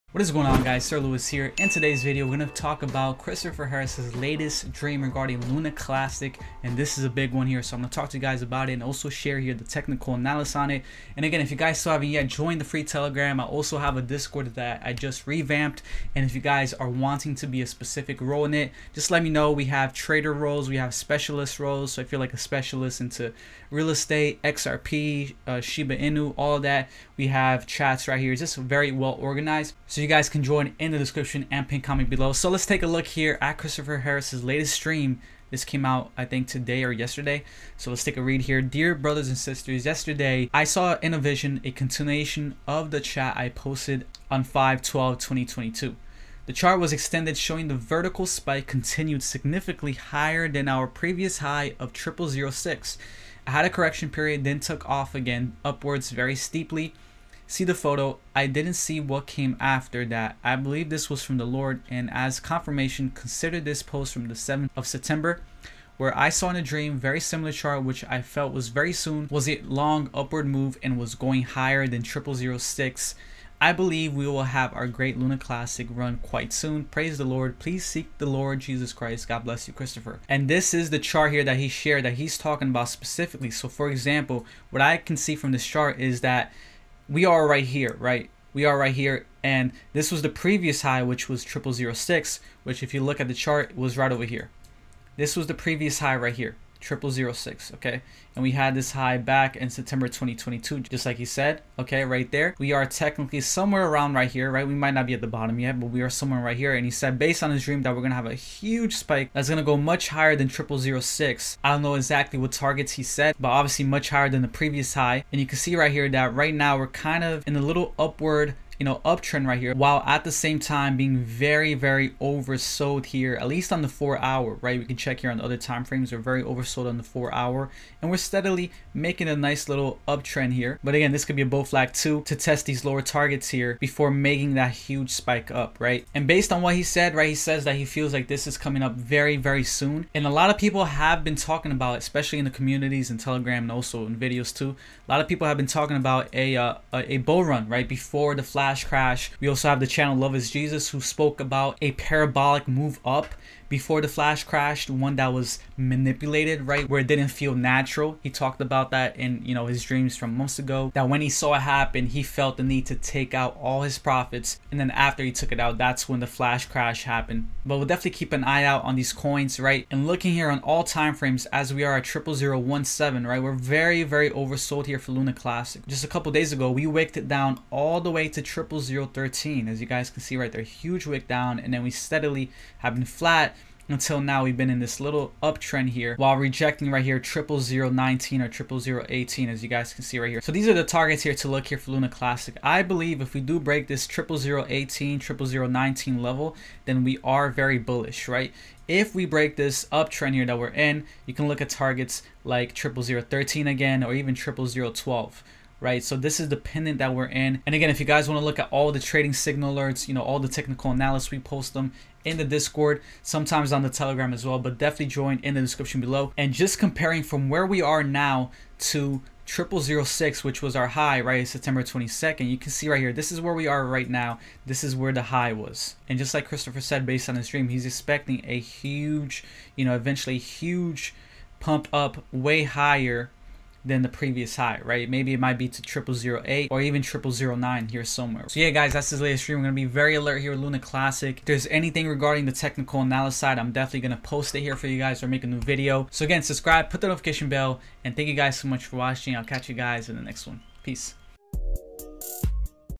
New Luna Classic Dream From Christopher Harris To Pass Previous High
New Luna Classic Dream From Christopher Harris To Pass Previous High In this video I talk about the latest dream from Christopher Harris Telegram. Nothing in this video is financial advice please make your own decisions and research.

~ Sir Luis 💚
My Equipment 🔰
My Camera 📷
My Microphone 🎤
My Headphones 🎧
My Monitor 💻
My Table ✍